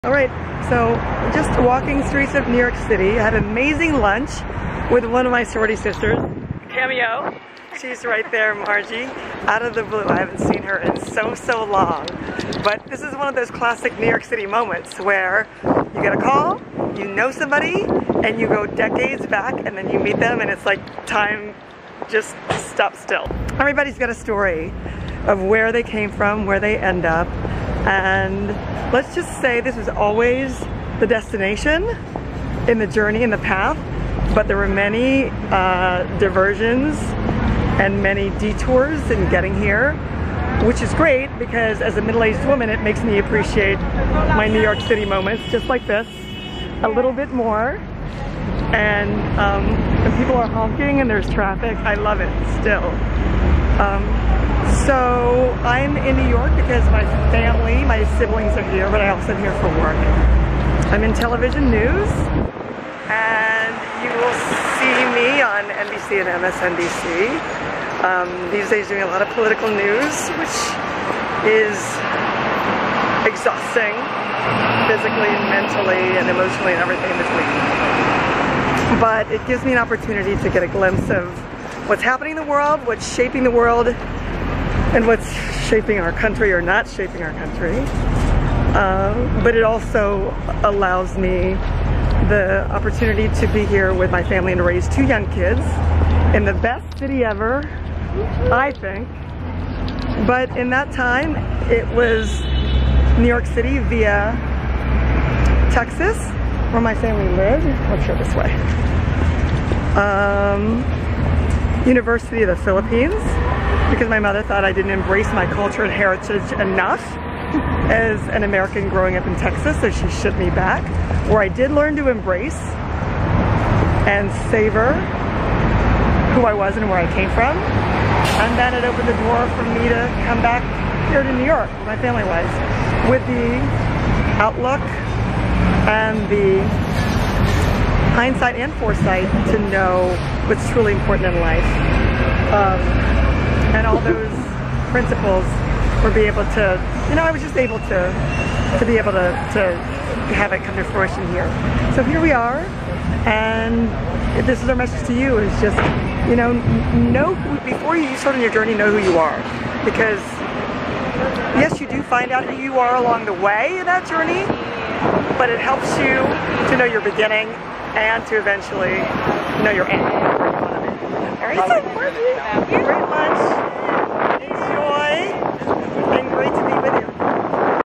FRANCES RIVERA Reporting From NYC
MSNBC and NBC News anchor lands in New York City via Texas and the University of the Philippines, where "I did learn to embrace and savor who I was and where I came from." Frances' message: "Before you start on your journey, know who you are."
Follow her on Instagram: francesrivera